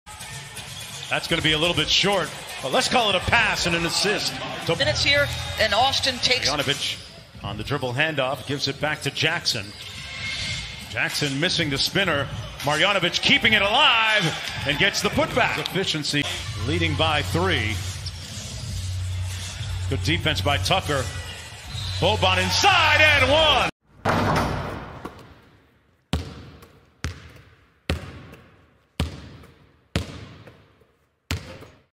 Boban Marjanovic 6 Pts 5 Reb Houston Rockets vs Dallas Mavericks Highlights NBA Restart
Boban Marjanovic 6 Pts 5 Reb
Houston Rockets vs Dallas Mavericks  153  149